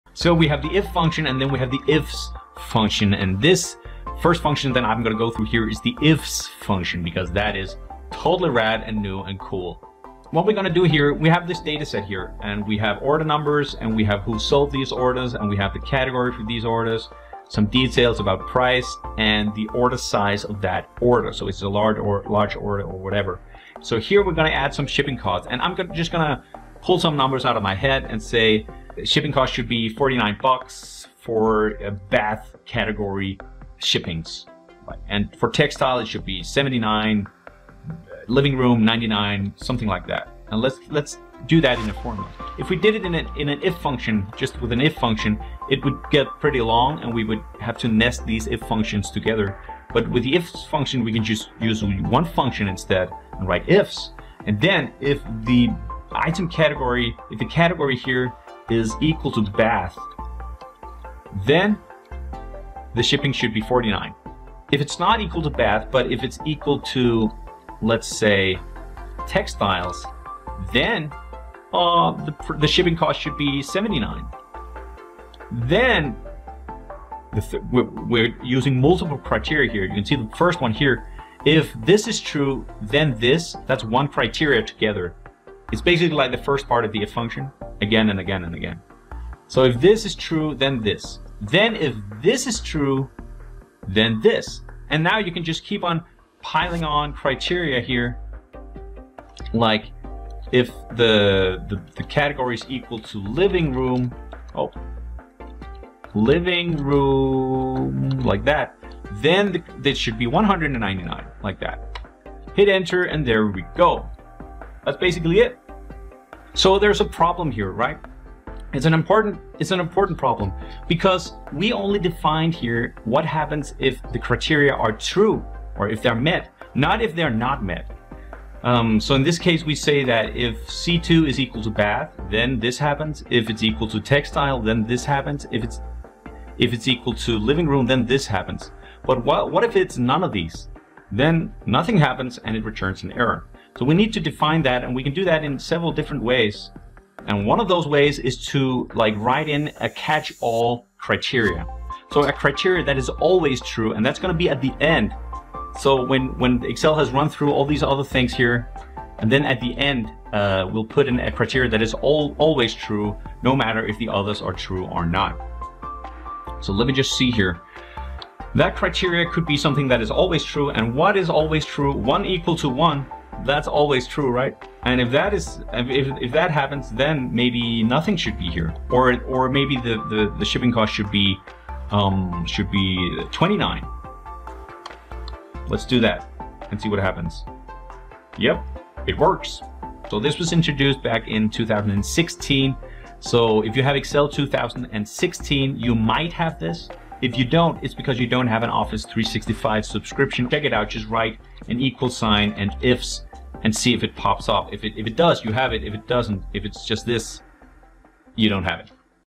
IF with multiple criteria (using the IFS function)
Replace (some) nested IF functions with the "new" IFS function - including examples and how to troubleshoot.

🕒 TIMESTAMPS
0:00 How to use IFS
1:55 How to troubleshoot IFS